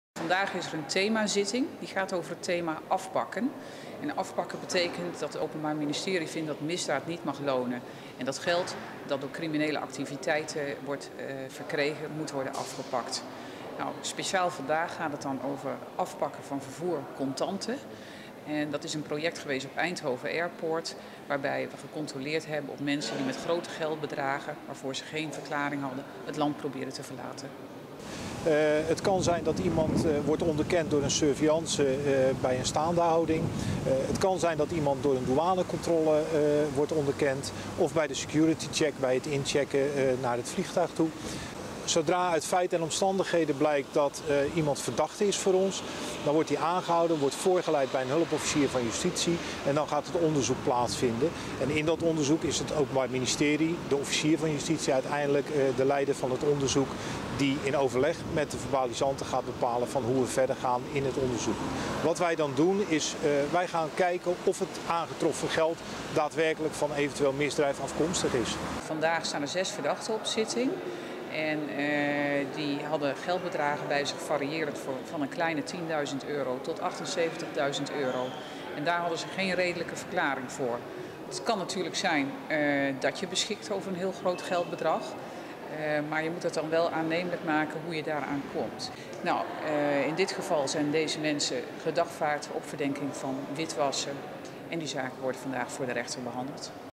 Afpakken op Eindhoven Airport
Op 21 januari 2014 staan voor de rechtbank Oost-Brabant tijdens een themazitting 'Afpakken' 8 personen terecht die op Eindhoven Airport zijn aangetroffen met grote sommen geld waarvoor ze geen verklaring hadden. De geldbedragen variëren van 10.000 tot 78.000 euro vertelt officier van justitie Juliëtte Nederlof. Een belangrijk speerpunt van het Openbaar Ministerie is dat geld dat door criminele activiteiten wordt verkregen, moet worden afgepakt.